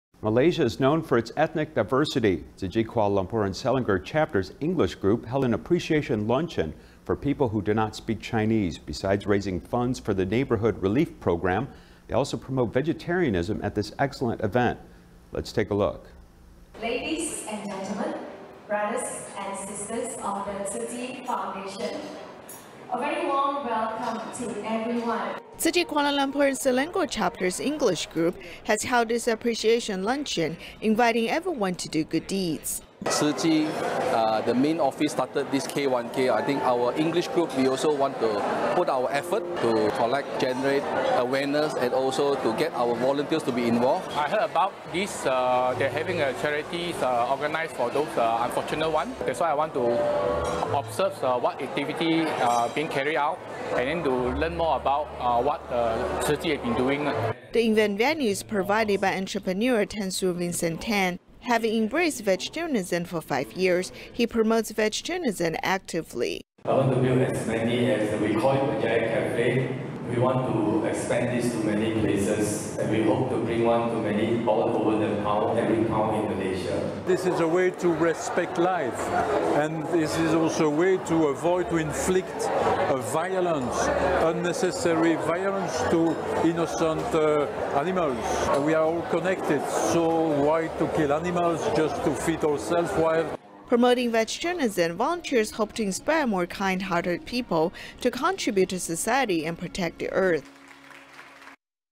20201020 Appreciation luncheon promoting vegetarianism
Malaysia is known for its ethnic diversity. Tzu Chi Kuala Lumpur and Selangor Chapter's English group held an appreciation luncheon for people who do not speak Chinese. Besides raising funds for the neighborhood relief program, they also promote vegetarianism at this excellent event.